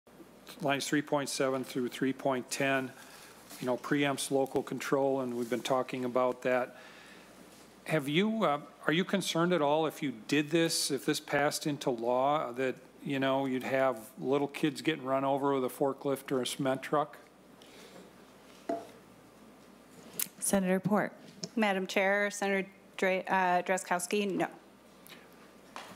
Minnesota Republican asks if 'little kids will be ran over by cement trucks' during bill discussion
Minnesota State Sen. Steve Drazkowski (R), on a bill allowing new affordable housing in commercial zones: "Are you concerned at all if this passed into law that you'd have little kids getting run over with a forklift or a cement truck?"

Sen. Lindsey Port (DFL): "No."